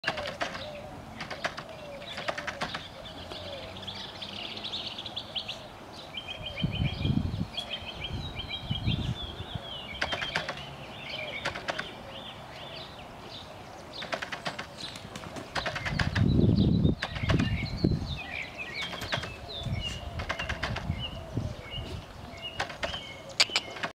Cocoa
Cocoa enjoys playing with a toy in the morning sun!